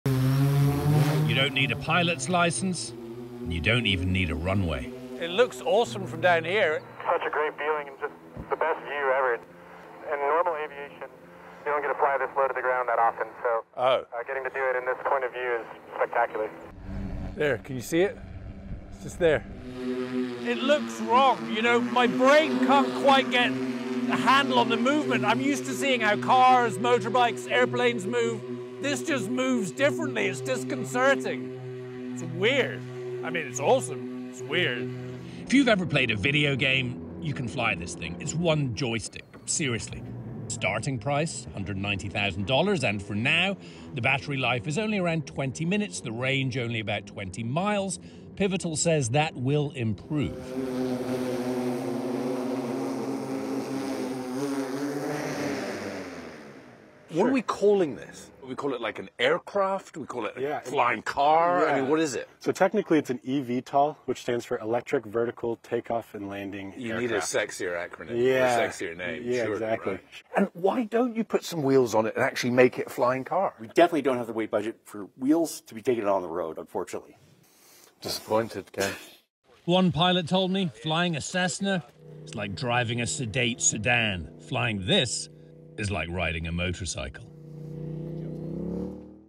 مفهوم امتلاك مركبة طائرة شخصية أصبح أمرًا حقيقيًا
أصبح مفهوم امتلاك مركبة طائرة شخصية أمرًا حقيقيًا، بعد ما قامت به شركة  "Pivotal" بسيارتها الطائرة والتي يبدأ سعرها من 190 ألف دولار.

ولقيادة هذه المركبة لا توجد حاجة لامتلاك رخصة قيادة طائرة، ولا حتى إلى مدرج إقلاع.

ورغم أن عمر البطارية لا يتجاوز 20 دقيقة، والمدى حوالي 20 ميلًا فقط، إلا أن شركة “Pivotal” تقول إن هذا سيتحسن مستقبلًا..

مراسل CNN، نِك وات، ينقل لكم التفاصيل في هذا التقرير.